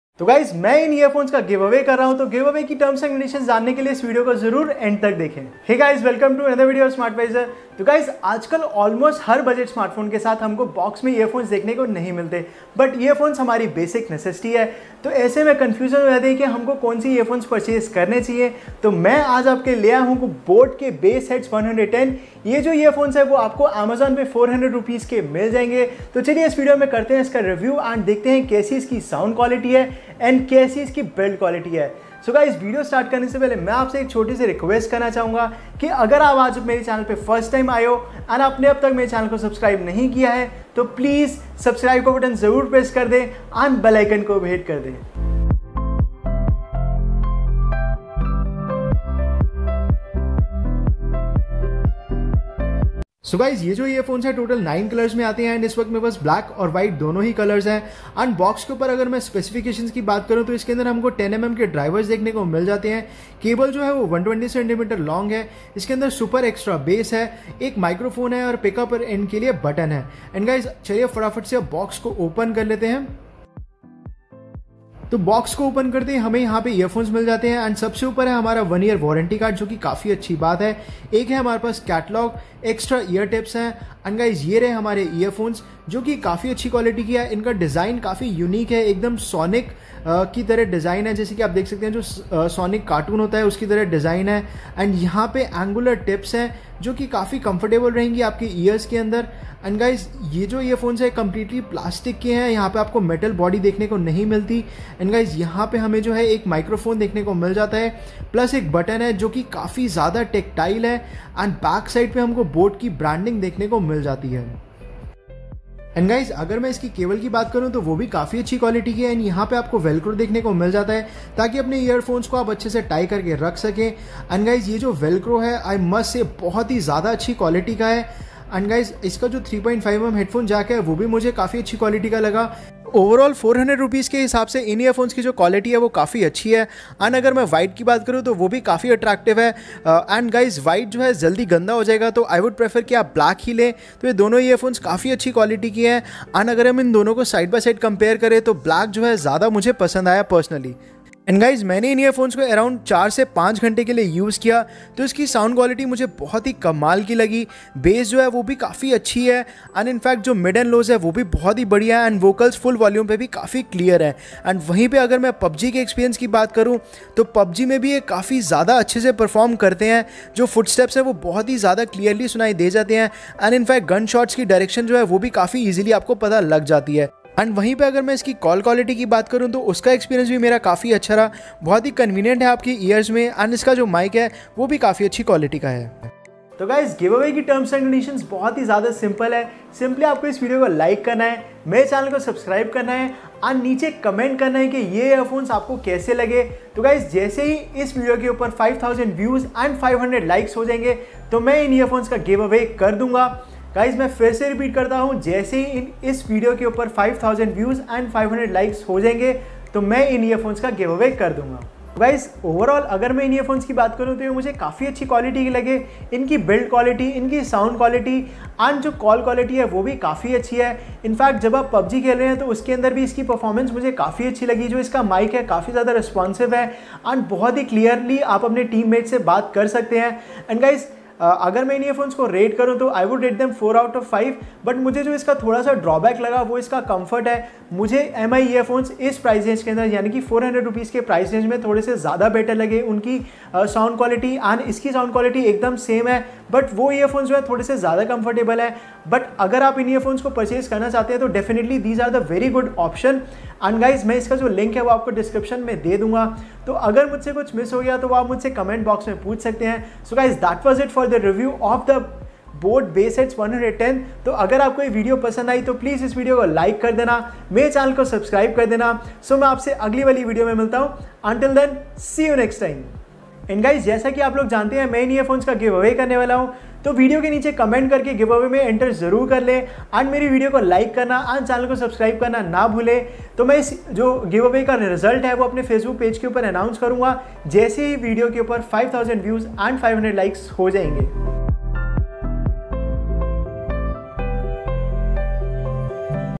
Boat Bassheads 110 wired earphones...! Best in Rs. 399 | Giveaway 🔥🔥🔥
Boat Bassheads 110 Review and Unboxing wired earphones in Rs. 399.

Galaxy Tab A 10.1
Galaxy Buds
Skullcandy Ink'd Wireless
Amazon Basics Tripod
DJI Osmo Pocket
GoPro Hero 7 (Silver)
Boya M1 Mic